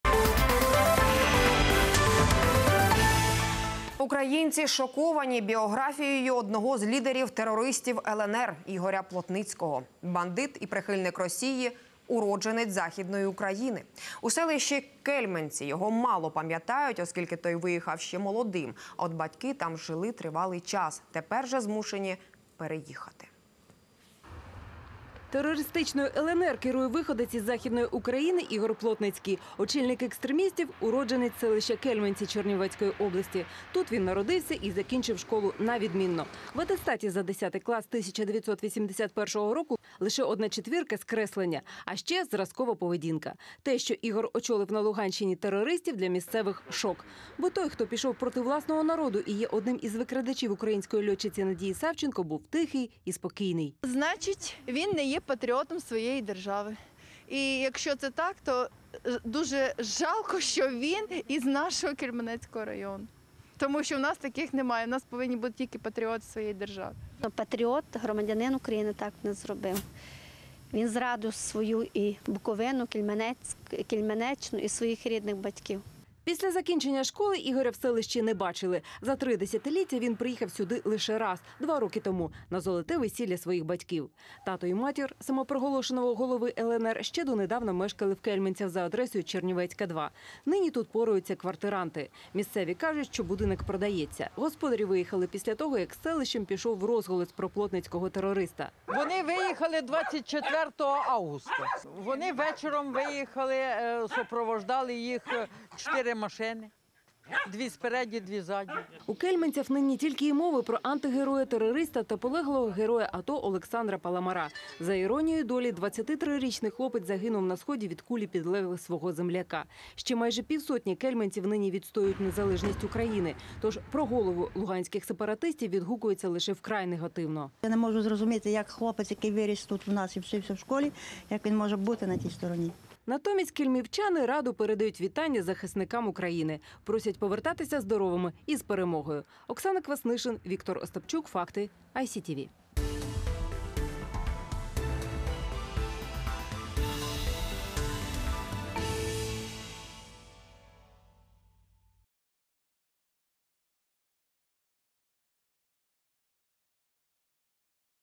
Шокирующая биография главаря "ЛНР" Плотницкого
Украинцы шокированы биографией одного из лидеров террористов "ЛНР" Игоря Плотницкого. Бандит и сторонник России - уроженец Западной Украины. Подробнее - в сюжете.

Українці шоковані біографією одного з лідерів терористів "ЛНР" Ігоря Плотницького. Бандит і прихильник Росії - уродженець Західної України. Детальніше - у сюжеті.